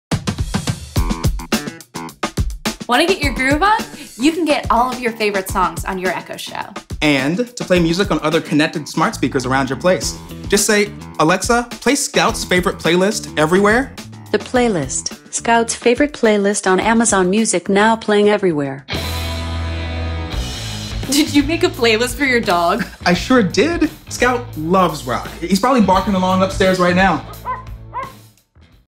Play Your Favorite Songs Everywhere
In this video, you’ll learn how to use Alexa and your Echo Show, Echo Show 5 and Echo Show 8 to help make life easier like playing your favorite songs everywhere.

Use these tips on the following Alexa devices:
Echo Show
Echo Show 5
Echo Show 8

Explore More with Alexa